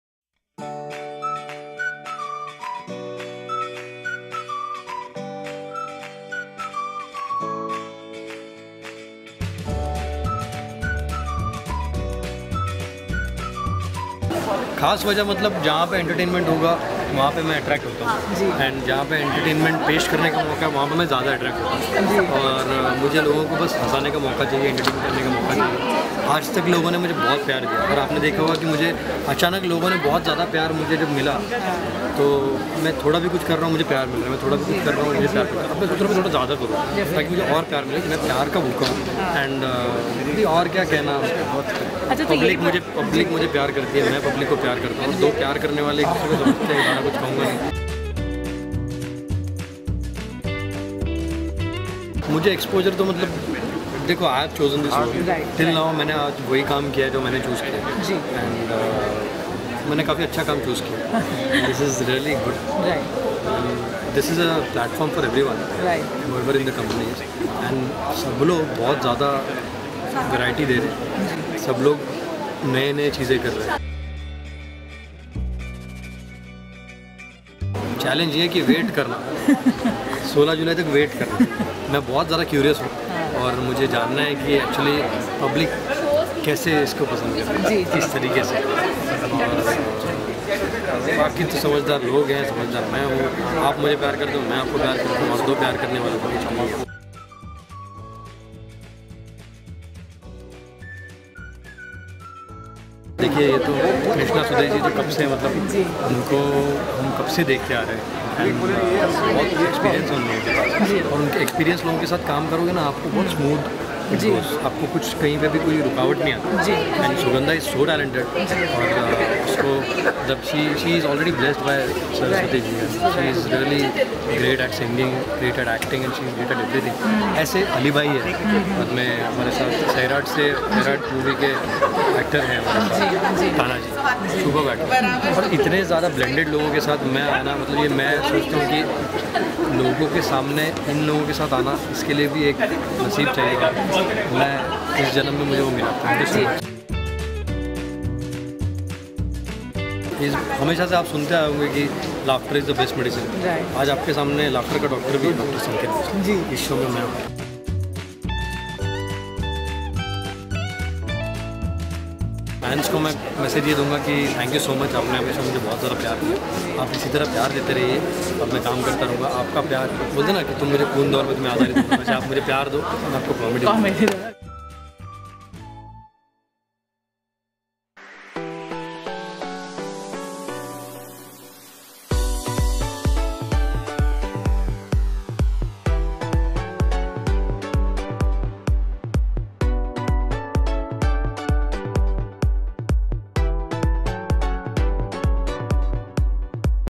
Sanket Bhosale talks about his NEW COMEDY SHOW | The Drama Company | Sony TV | Exclusive
Known for his comedy skills/timing and mimicry , comedian Dr.Sanket Bhosale talks about his new show , chemistry with co-stars and MORE!